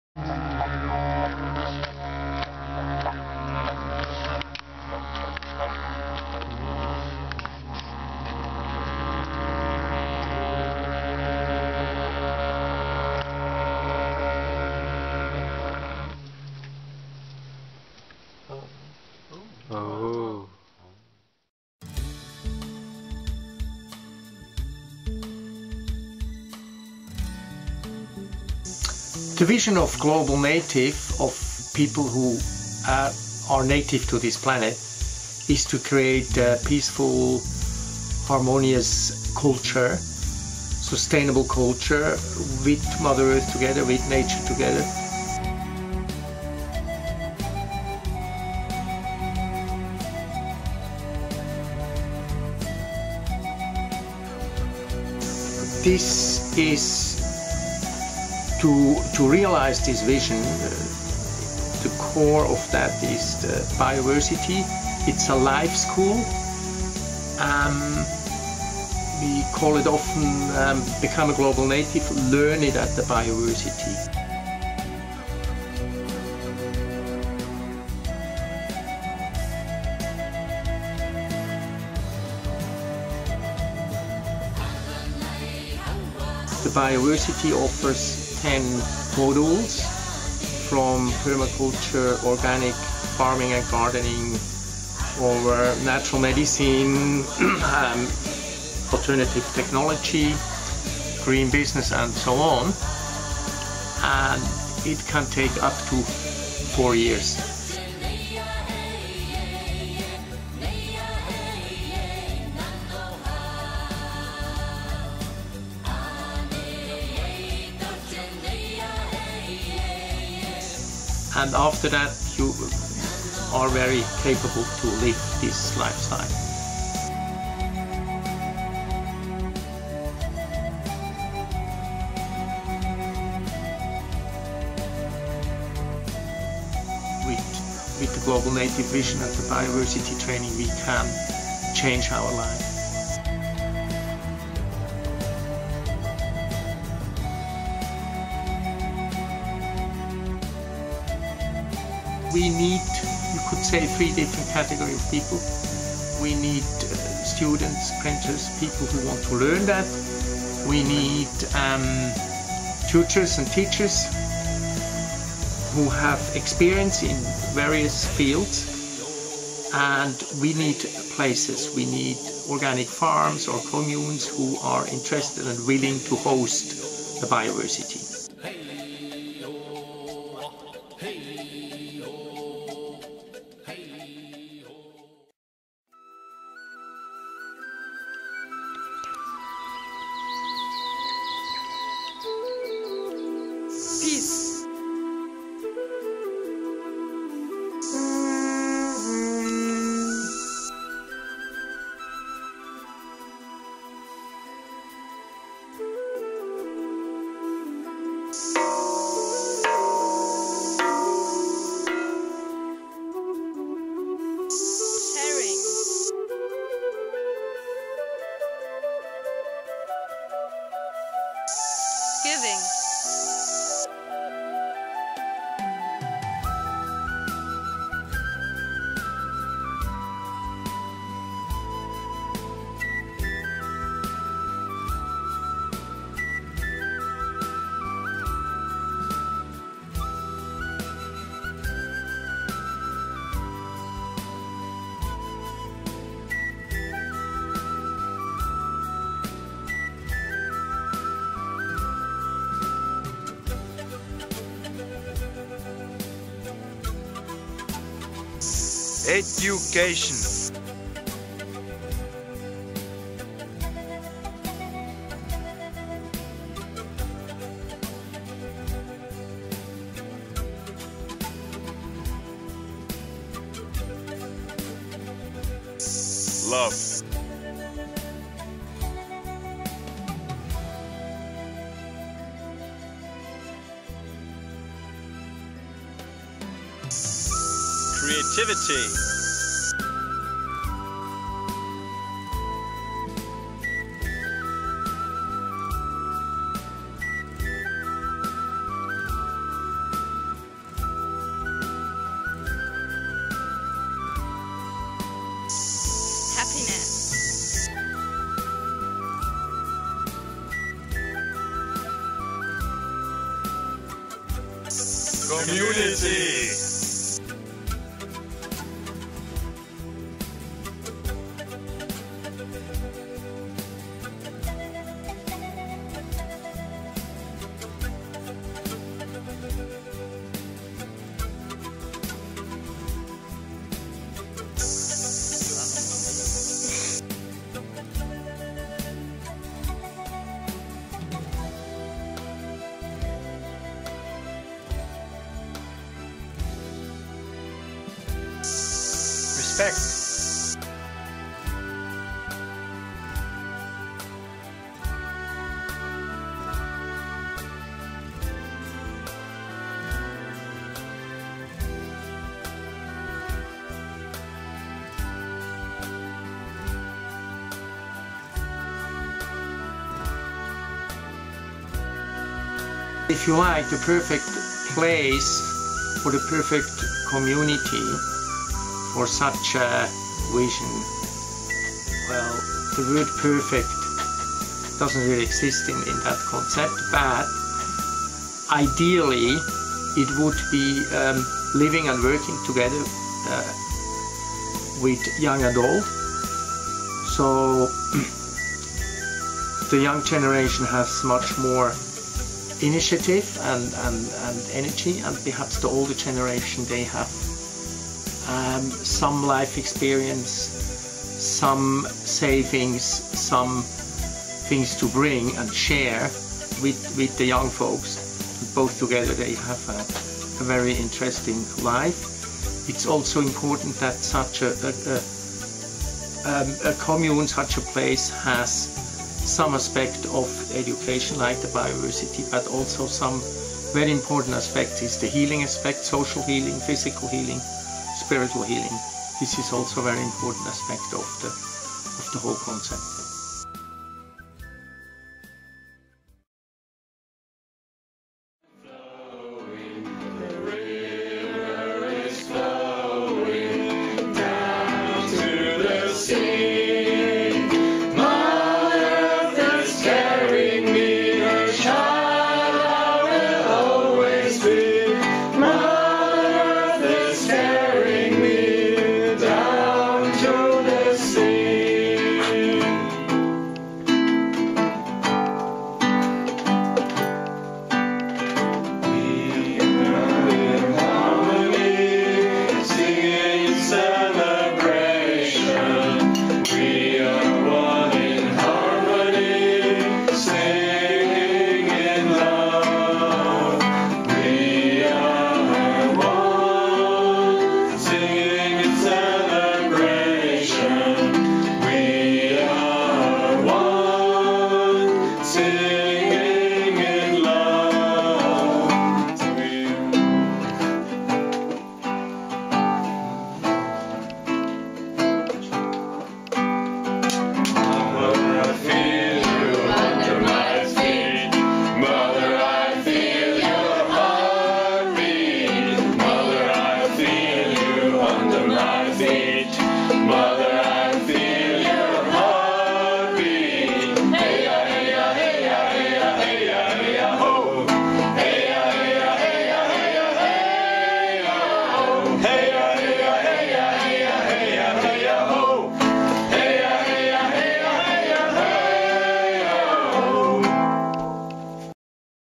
Global Native Golden Bay, New Zealand 2014
Become a Global Native.  Learn it at the Bioversity. This is a video from our Startup Camp 2014 in Golden Bay, New Zealand.